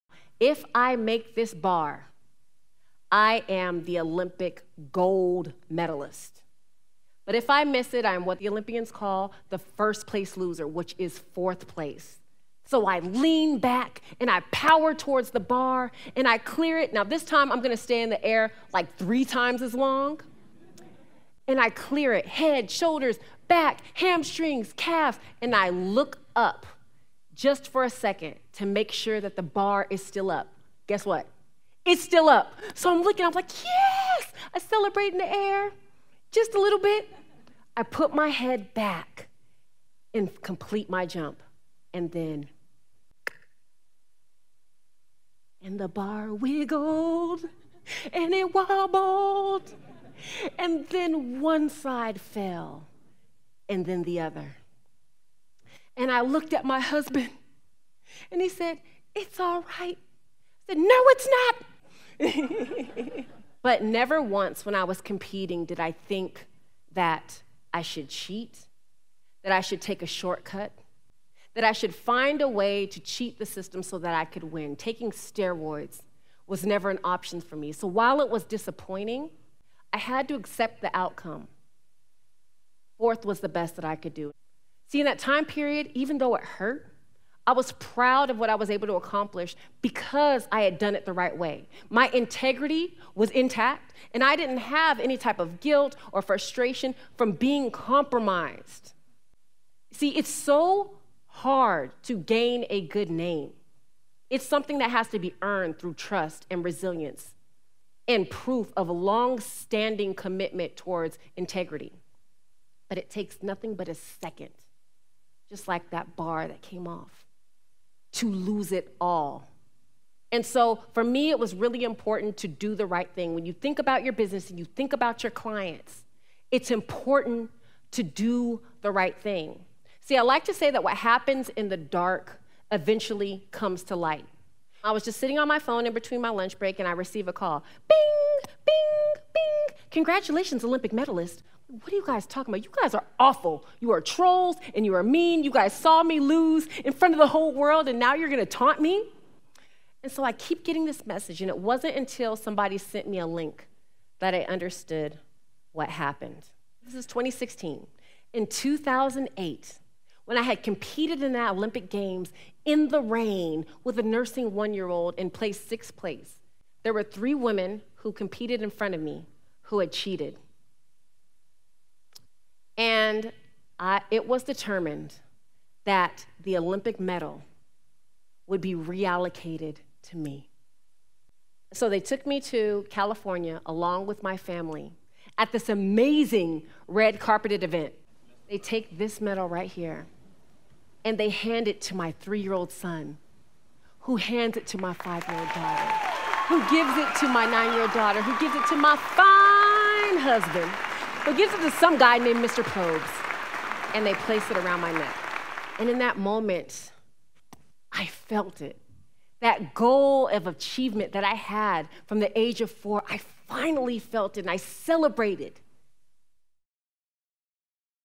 Doing the right thing pays off
When Chaunté Lowe placed sixth in high jump during the 2008 Olympics, she wondered if that was the highest she could go.